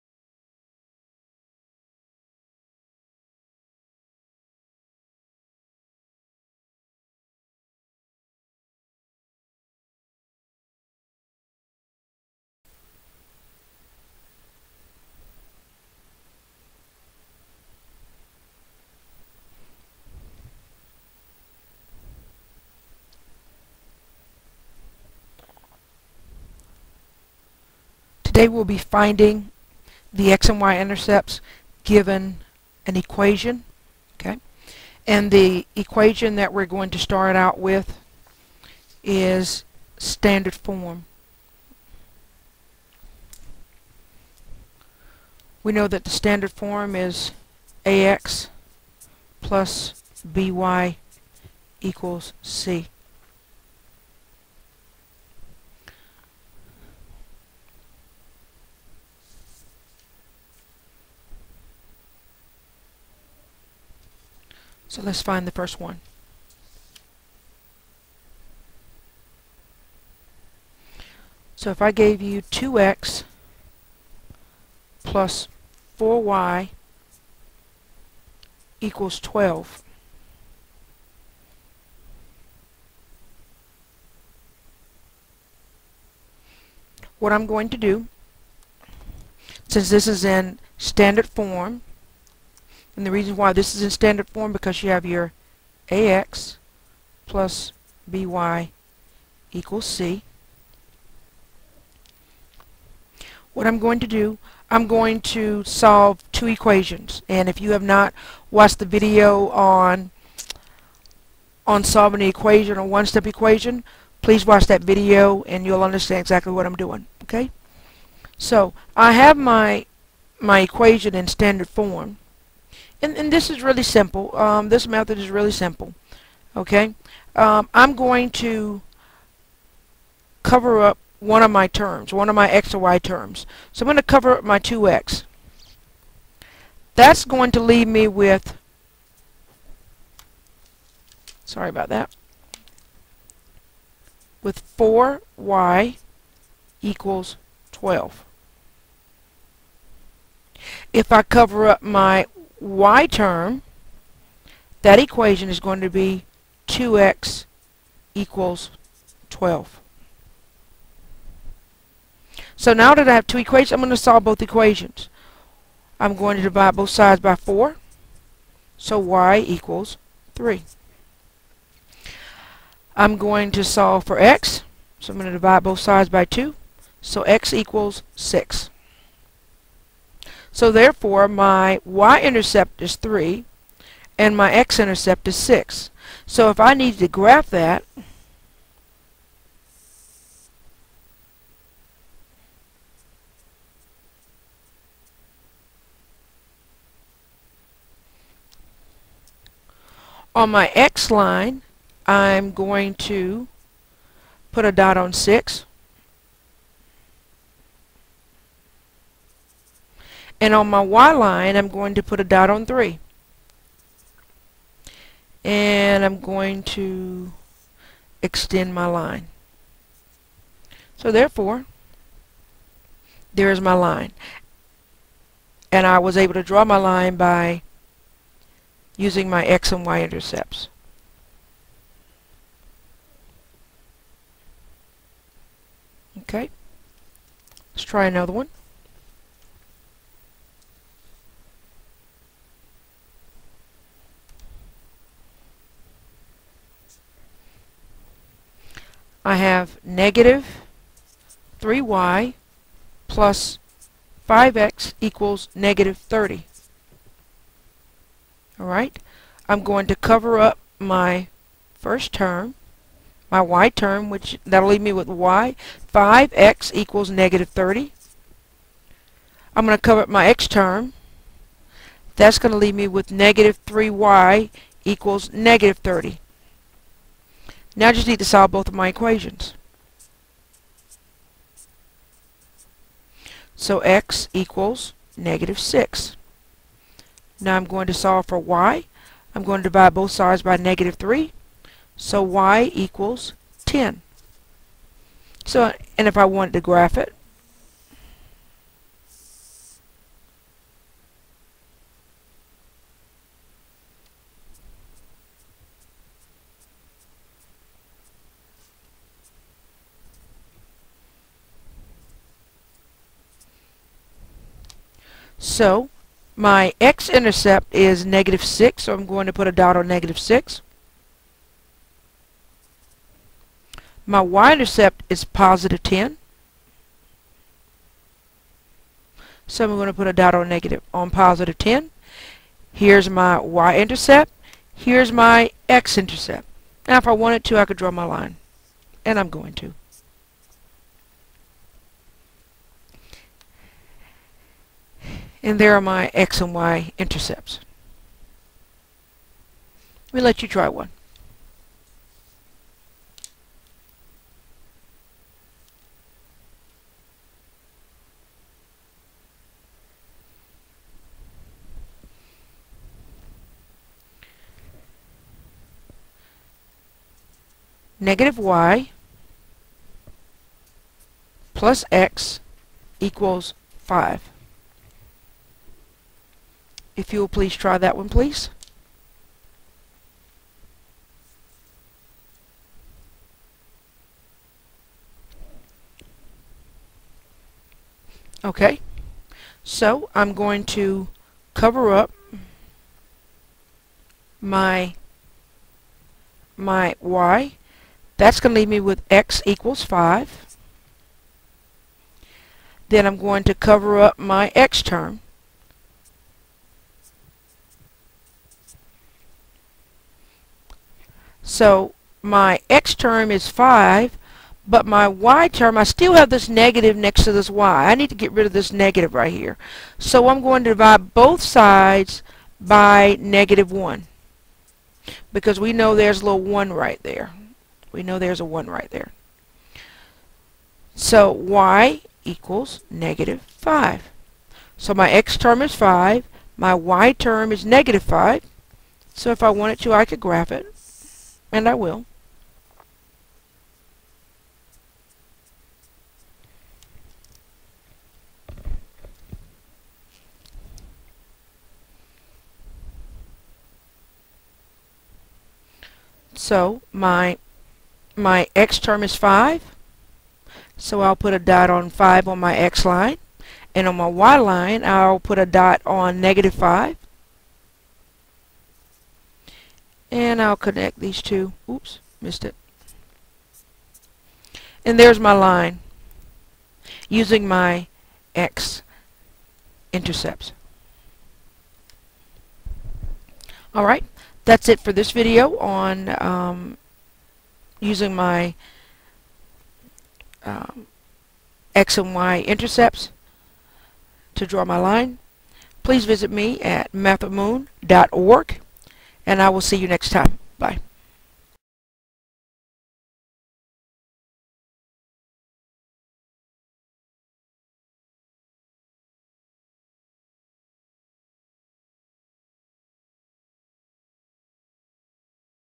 Find x and y intercepts given standard form - MathwithMoon.org
Find the x and y intercept given an equation in standard form